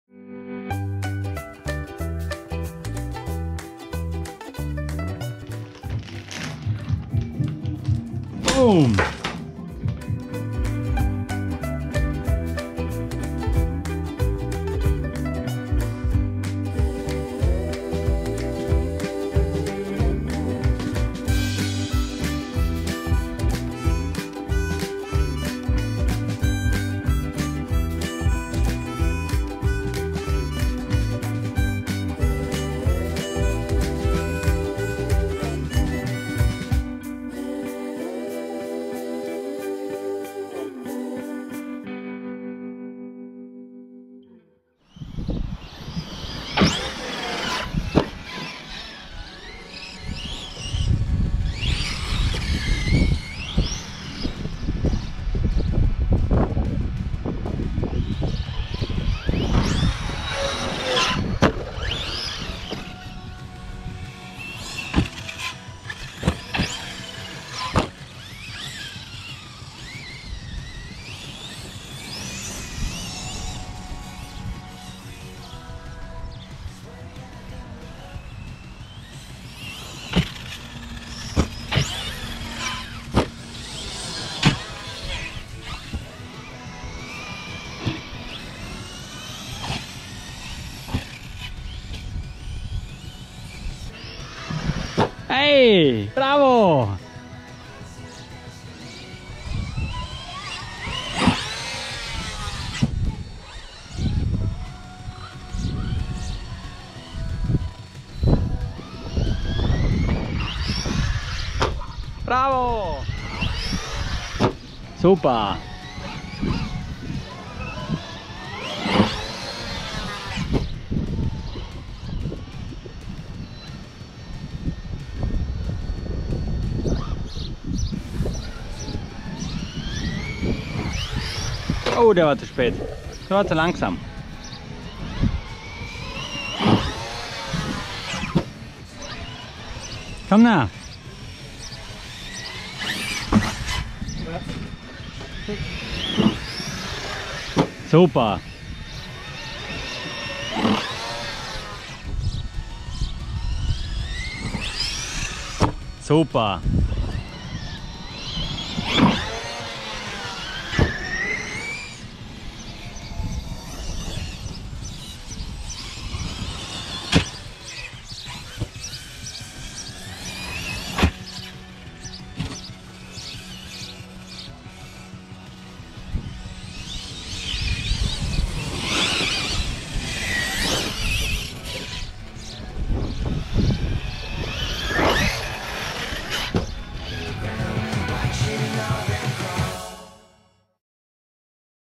10 Juni 2022 Backflip Freitag
Schrauben und Firstrun vom Kraton 6s EXB vom Bernhard.
Die Max6 Combo geht richtig gut.
Mein kleiner fährt auch schon mega 😍
Springt tolle Backflips mit dem MT410.
Ich bin mega stolz 👌